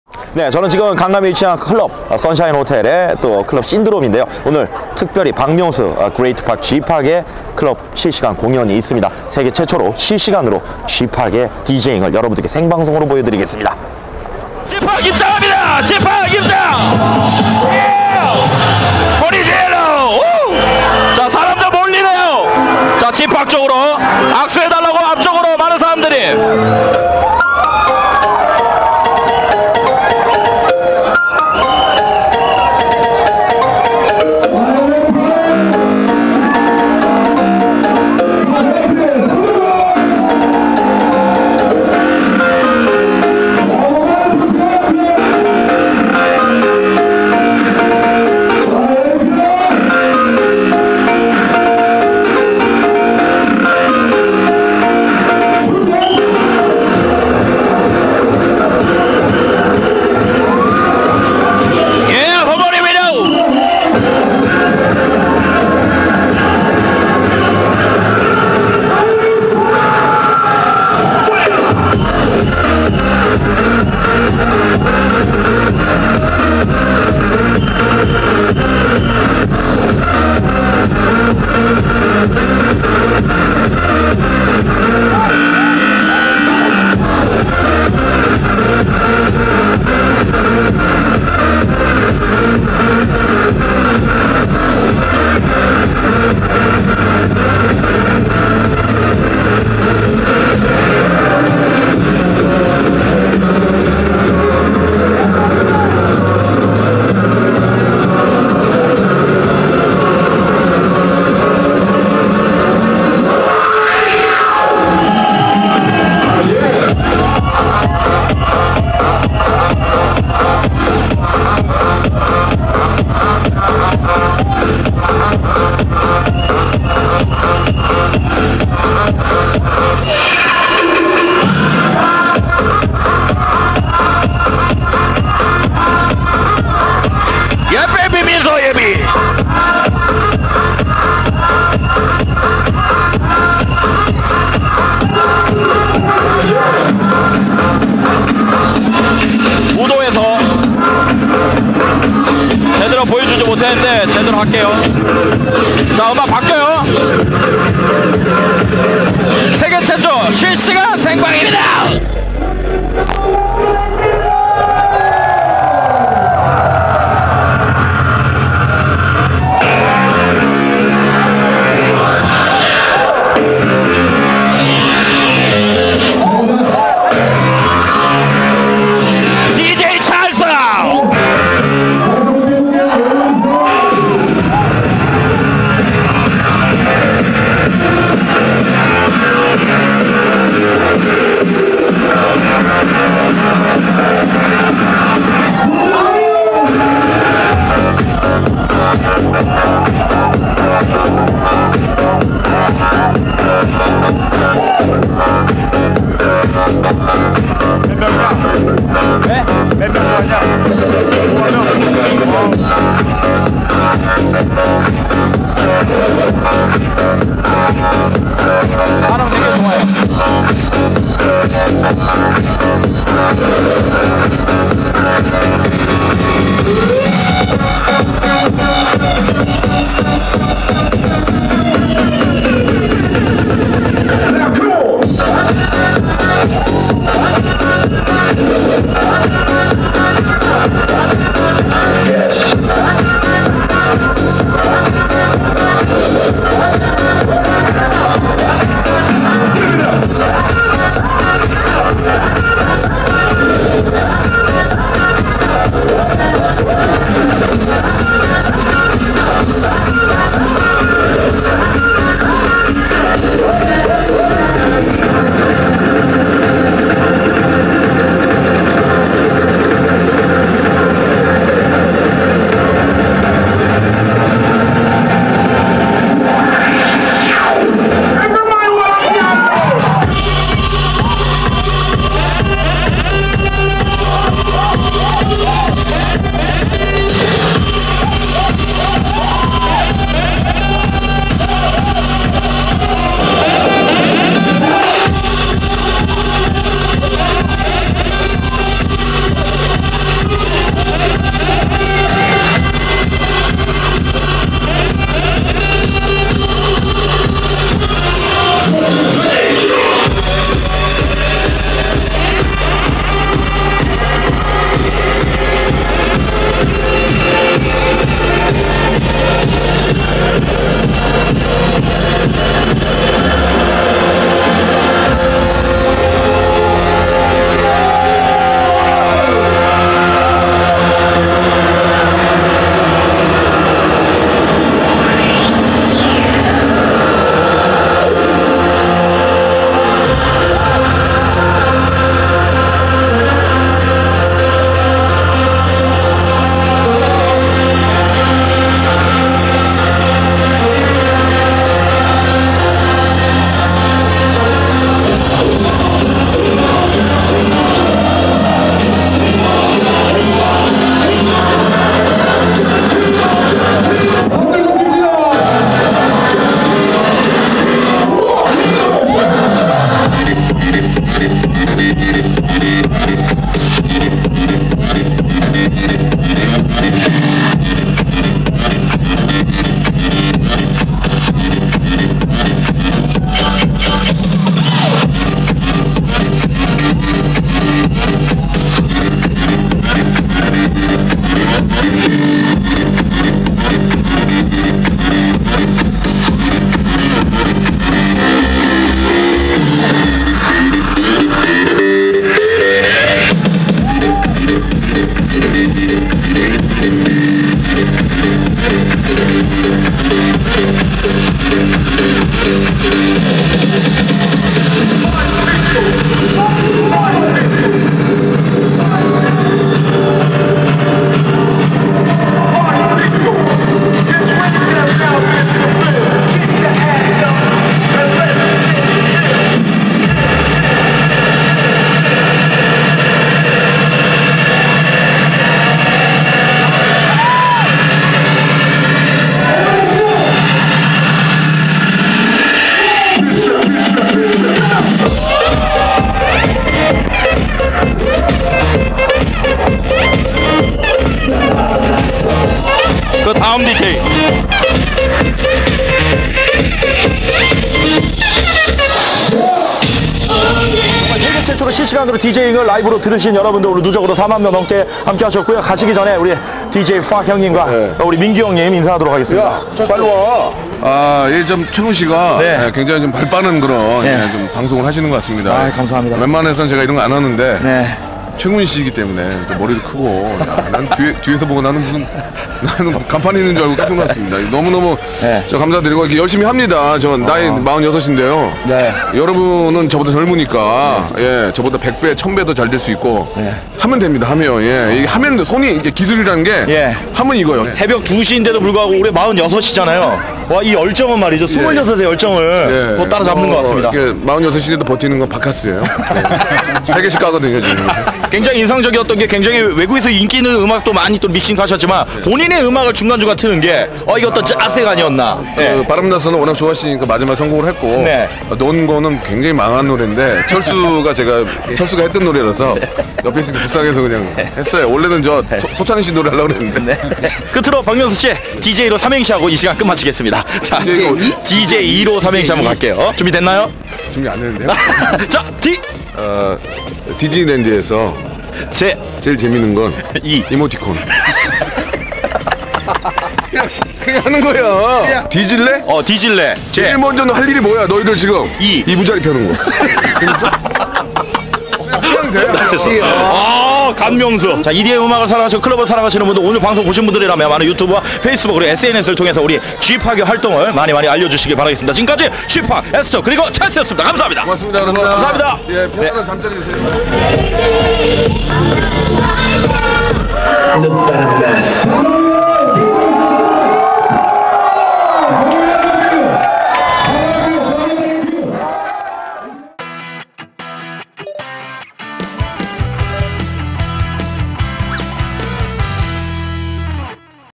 G-Park (박명수)의 CLUB DJing 현장!! [oh Hot] - KoonTV
G-Park (박명수)의 현란한 CLUB DJing 현장!!